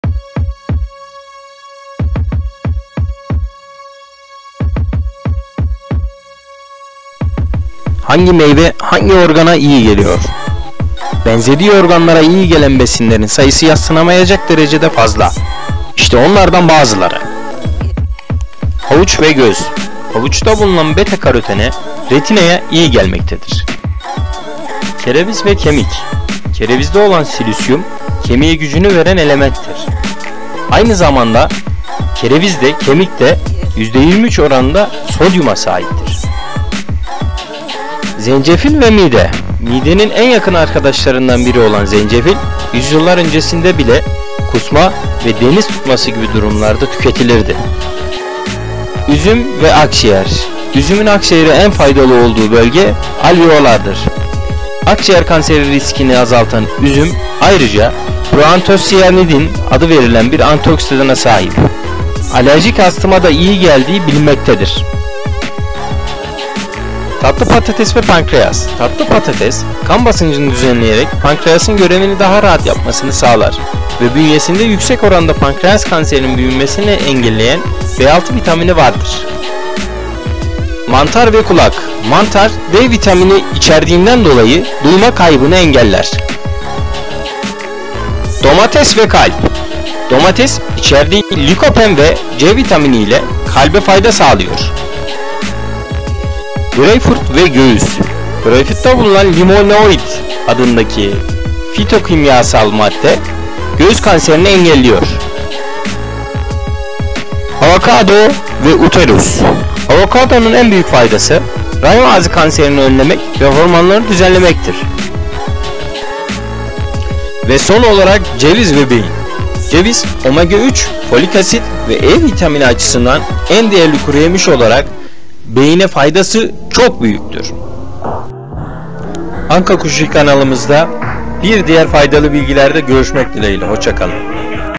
Hangi meyve hangi organa iyi geliyor? MEYVE ve ORGAN BENZERLİĞİ
Hangi meyve hangi organa iyi geliyor?
Benzediği organlara iyi gelen besinlerin sayısı yadsınamayacak derecede fazla. İşte onlardan bazıları.